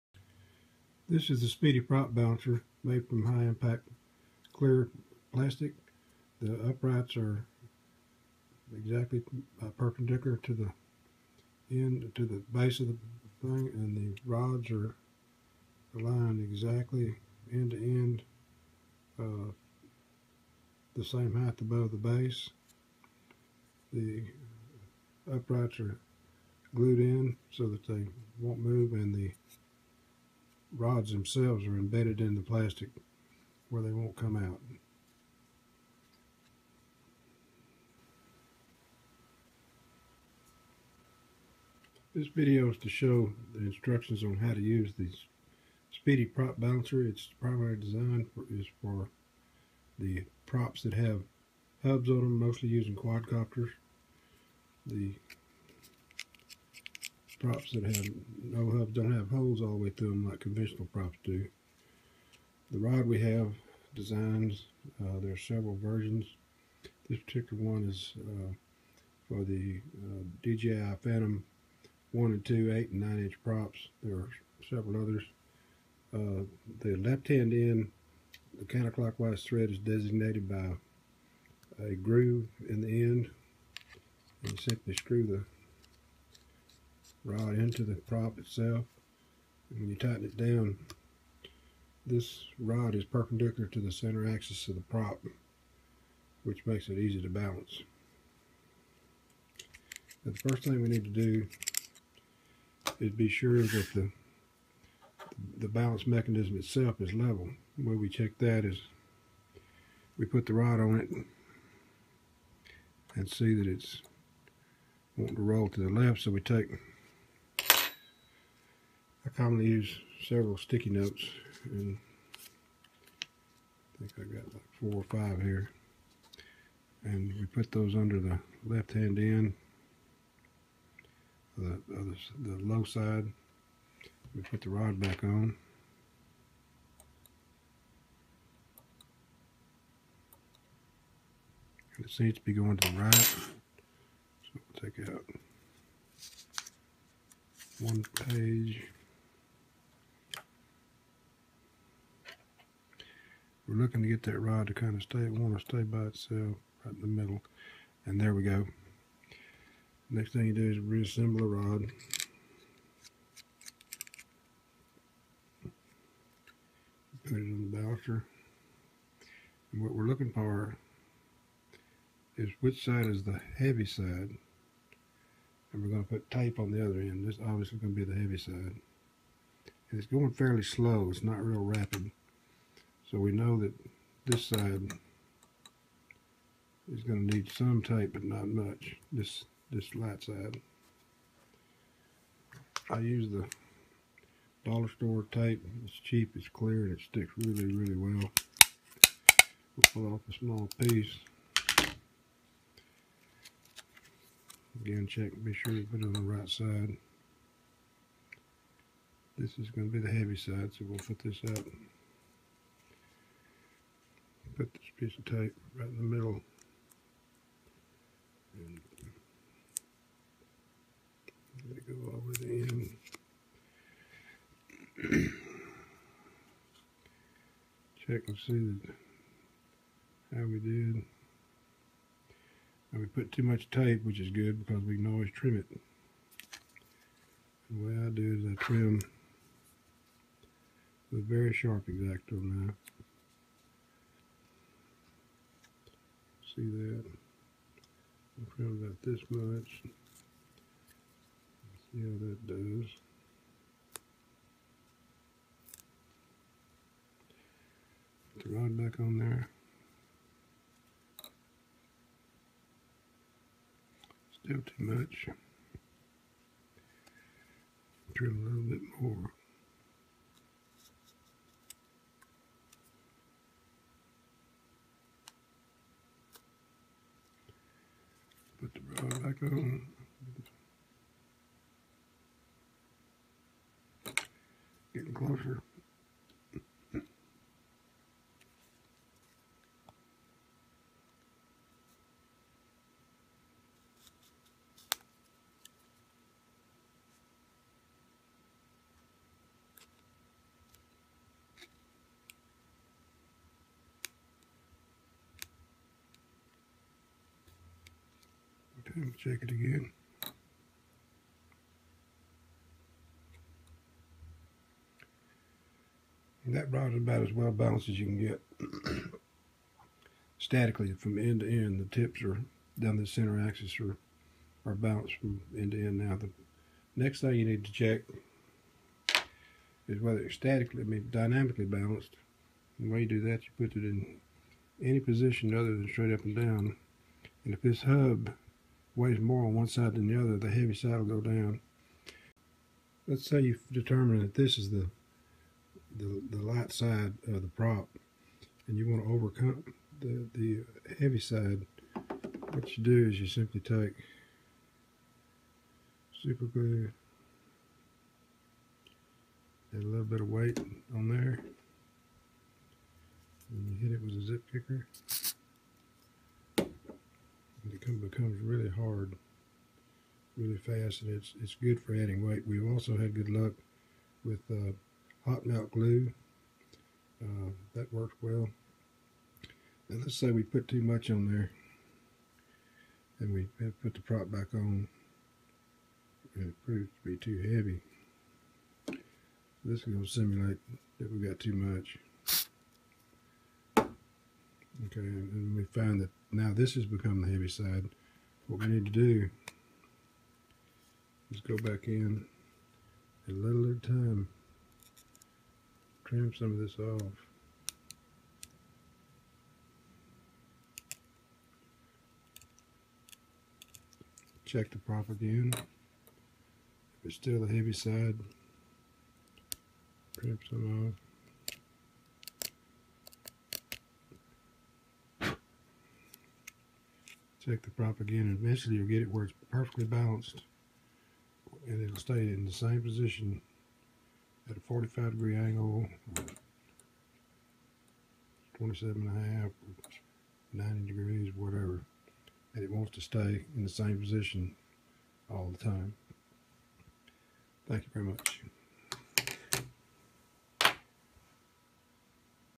Speedy Prop Balancer for Quadcopters
This video shows the features and benefits of using the Speedy Prop Balancer to balance hubbed quadcopter props to improve motor effeciency and eliminate vibrations in the frame that would show up in your videos.